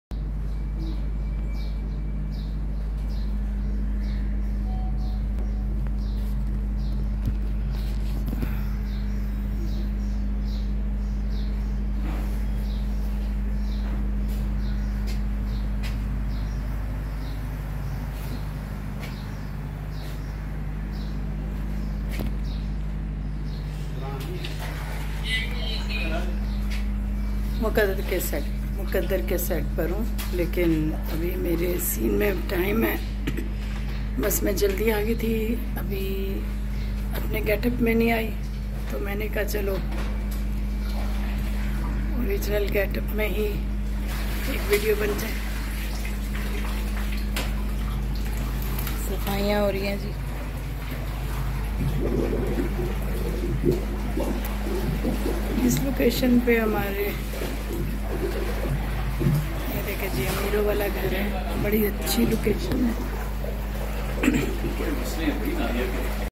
in real getup kyun kay abhi shooting start ni hoi socha chota sa vlog hojay
without getup kyun kay abhi shooting start ni hoi socha chota sa vlog hojay on set MUQADDAR